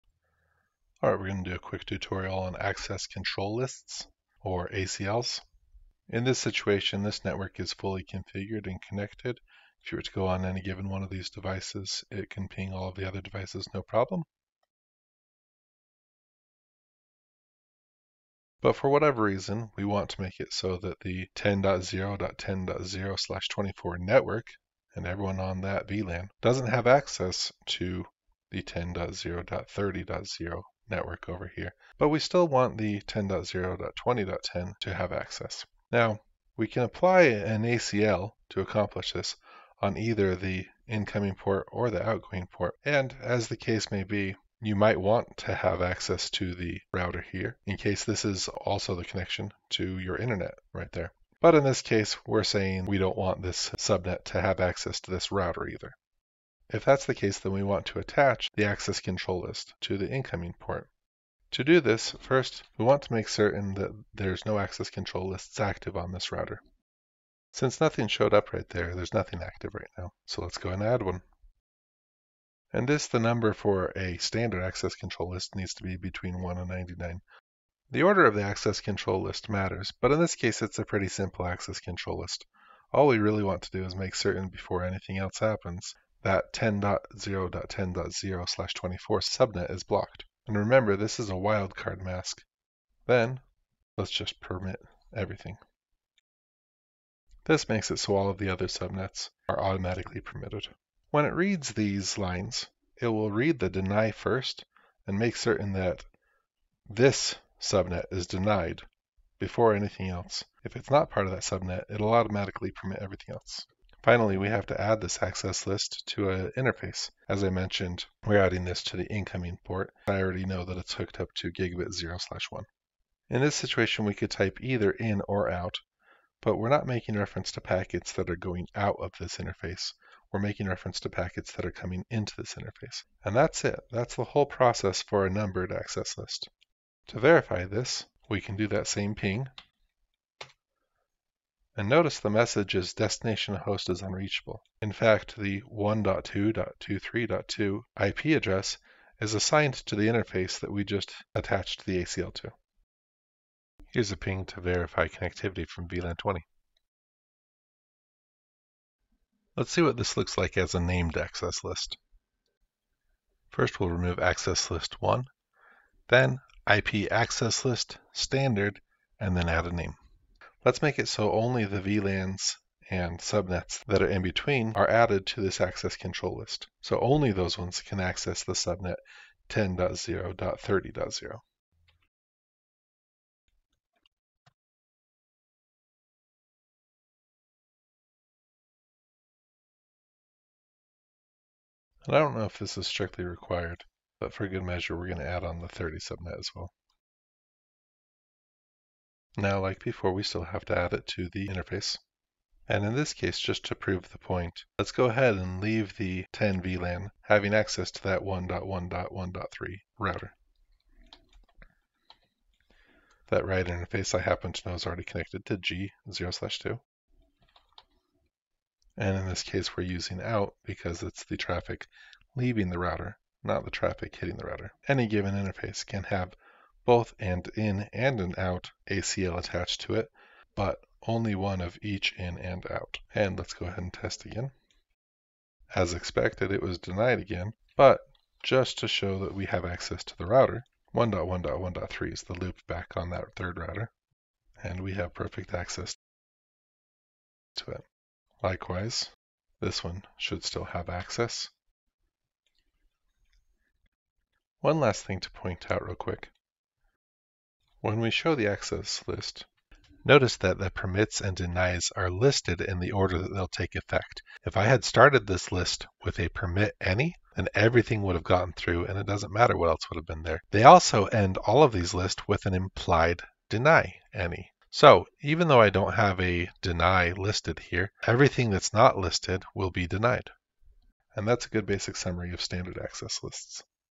IT Networking - Standard ACL - Access Control List
A very quick implementation of Standard Access Control Lists (ACLs) done in Cisco Packet Tracer.
This is one of the most basic elements of network security that ought to be implemented on every network.
This is intended to be a very simple implementation of an ACL, presented in a way that should be very easy for CCNA students to understand.